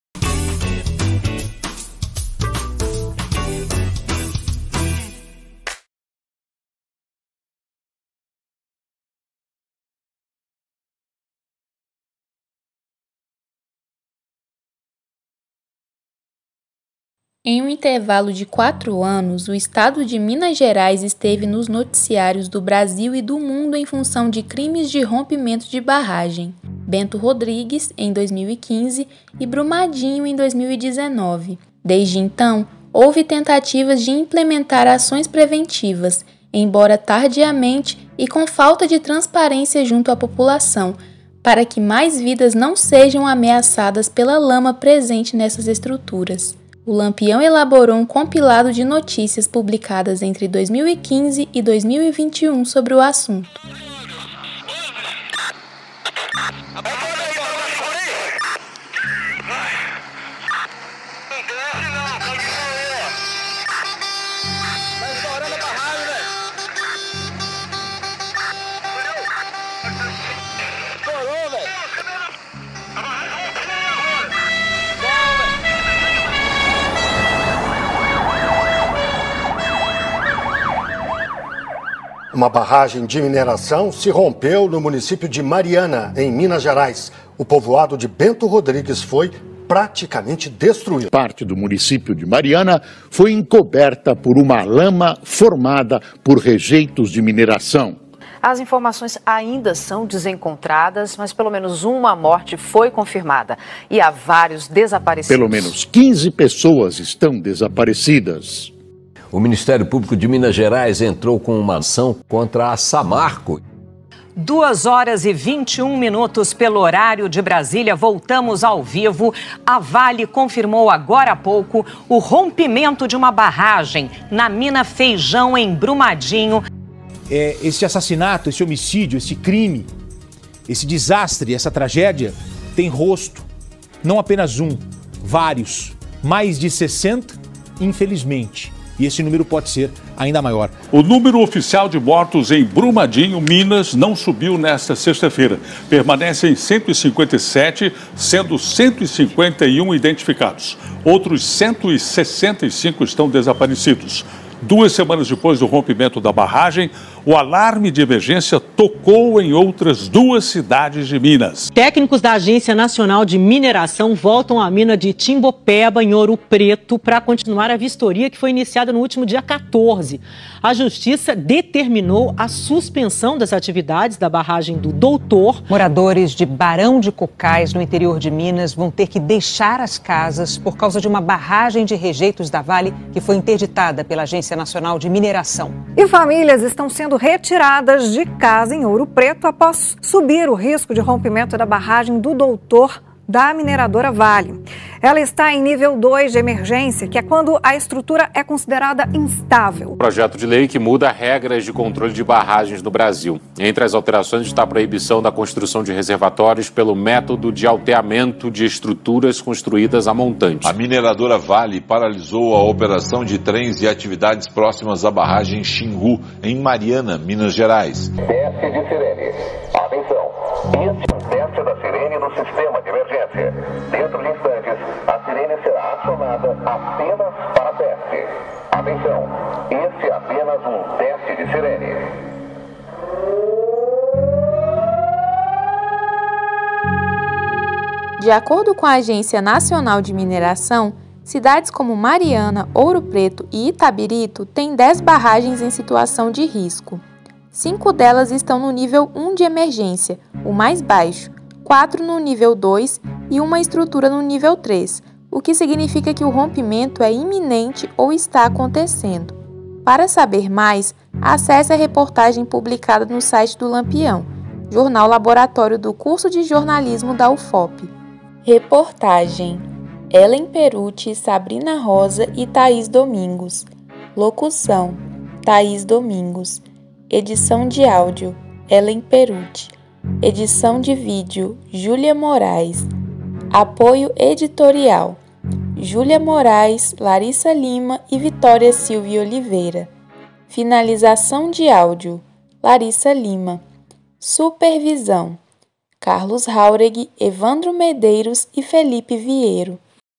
Produção Sonora - Barragens em Minas Gerais
Entre 2015 e 2019, crimes de rompimento de barragem em Minas Gerais estiveram nos noticiários do Brasil e do mundo e as consequências foram danos irreparáveis. Hoje, ações preventivas têm sido tomadas, embora não haja um acompanhamento transparente junto à população para que as tragédias não se repitam.

 Acompanhe as notícias publicadas entre 2015 e 2021 sobre o assunto.

Reportagem: Hellen Perucci, Sabrina Roza e Thaís Domingos

Locução: Thaís Domingos

Edição de áudio: Hellen Perucci

Edição de vídeo: Júlia Morais

Apoio editorial: Júlia Morais, Laryssa Lima e Victoria Silva e Oliveira

Finalização de áudio: Laryssa Lima

Supervisão: Carlos Jáuregui, Evandro Medeiros e Felipe Viero